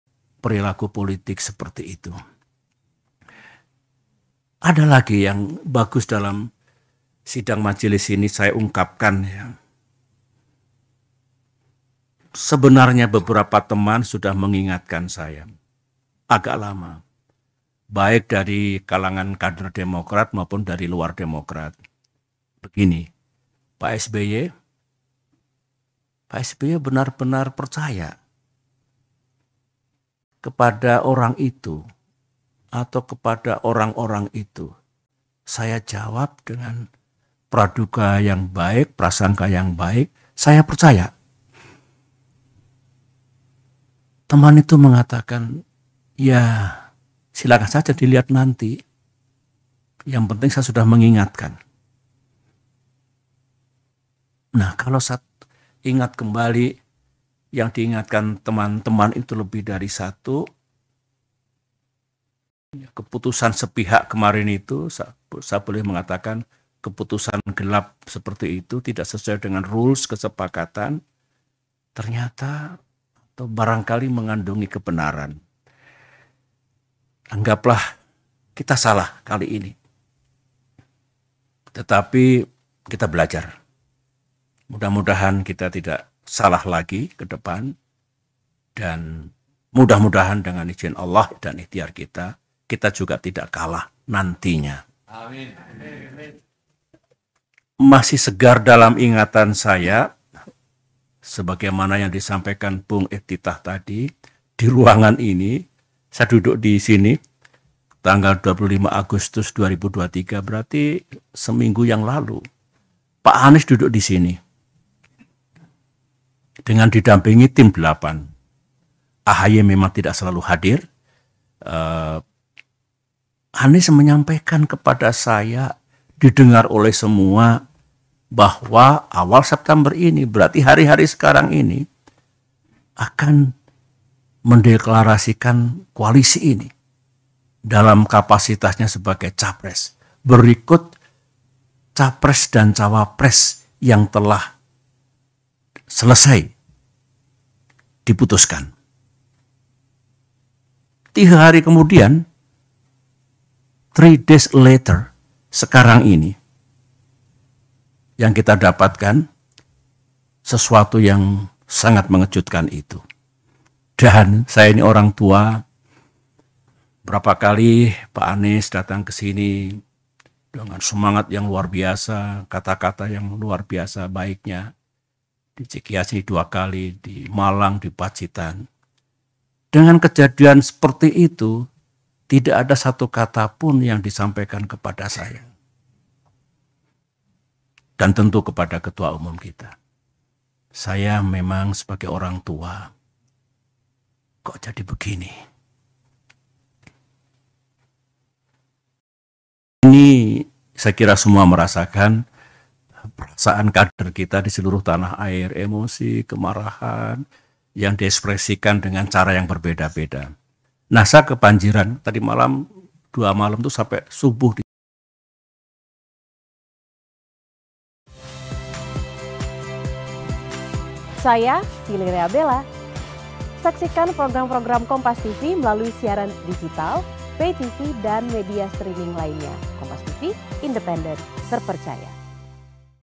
SBY Kecewa Tak Ada Kata yang Disampaikan Anies, Usai Koalisi Kandas
CIKEAS, KOMPASTV  Dalam konferensi pers di Cikeas, SBY mengulas ada pesan teman yang semula telah mengingatkannya terkait sosok Anies.

"Sebenarnya beberapa teman sudah mengingatkan saya, di kalangan Demokrat dan Pak SBY, benar-benar percaya kepada orang itu?, Saya jawab praduga yang baik saya percaya. Teman itu mengatakan, lihat saja nanti tapi saya sudah mengingatkan," ungkap SBY.

Baca Juga Anies Disebut Berkhianat, Begini Kata Juru Bicara Demokrat soal Komunikasi Terakhirnya dengan Anies di

Namun benar adanya usai adanya rilis tertulis terkait Anies menggandeng Muhaimin jadi cawapres, SBY kecewa tak ada kata dari Anies untuknya.

Hal itu diungkap SBY karena hari-hari sebelumnya pertemuannya dengan Anies berlangsung hangat dan menyatakan koalisi perubahan sebelumnya solid.

Saya ini orang tua, semangat yang luar biasa. Dengan kejadian seperti itu. Tidak ada kata yang disampaikan kepada saya," kata SBY.